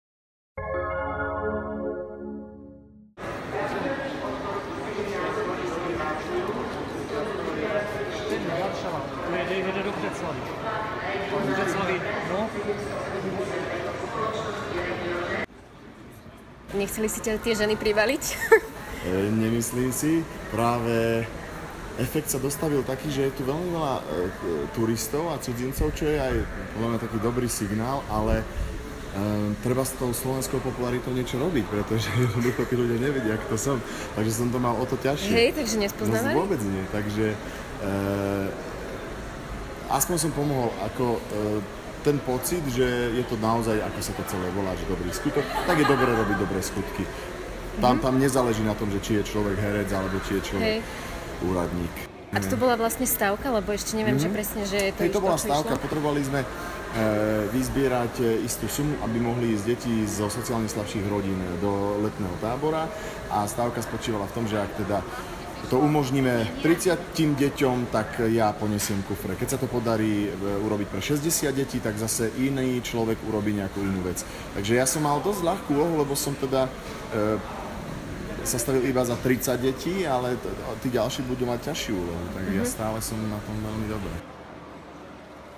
Z Koleníka sa stal nosič kufrov
Cestujúci na bratislavskej Hlavnej stanici zostali v šoku.